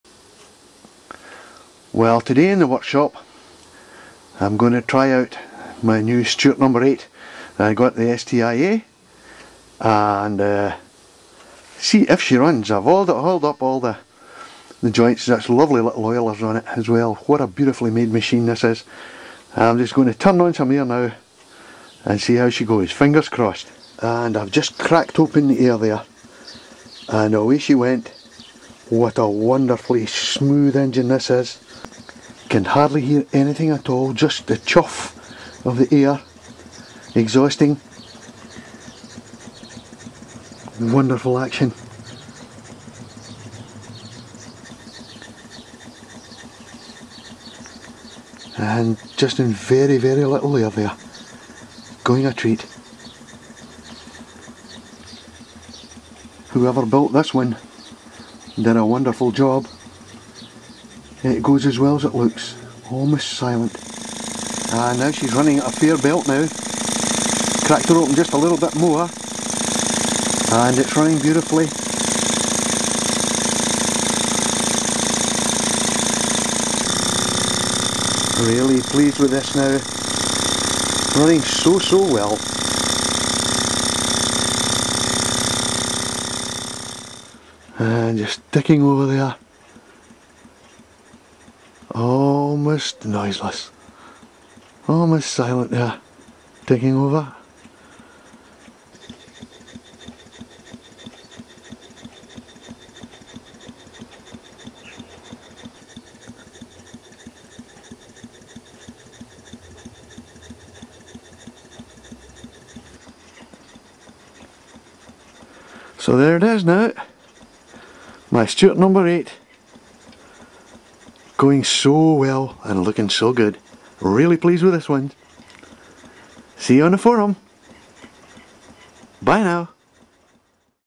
Stuart No 8 Model Steam Engine.
Stuart Live Steam Engine Running On Air.